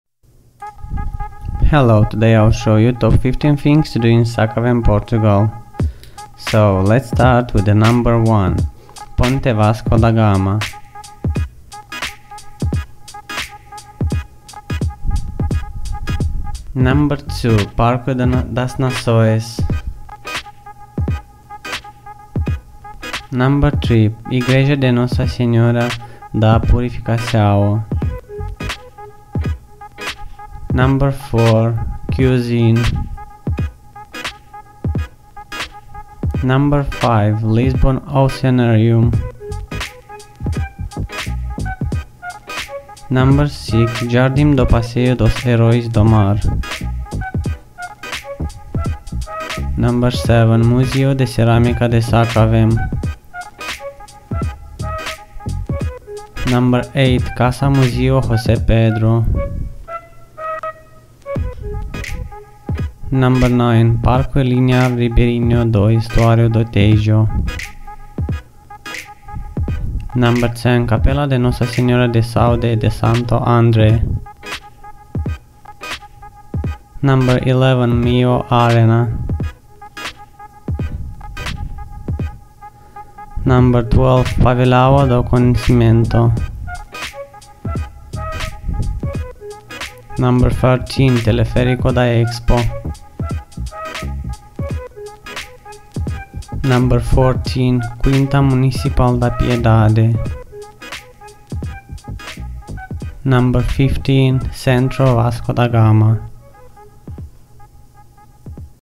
Top 15 Things To Do In Sacavem, Portugal
Here are top 15 things to do in Sacavem, Portugal
1. Ponte Vasco da Gama
3. Igreja de Nossa Senhora da Purificação
6. Jardim do Passeio dos Heróis do Mar
7. Museu de Cerâmica de Sacavém
9. Parque Linear Ribeirinho Do Estuario Do Tejo
10. Capela de Nossa Senhora da Saúde e de Santo André
14. Quinta Municipal da Piedade
15. Centro Vasco da Gama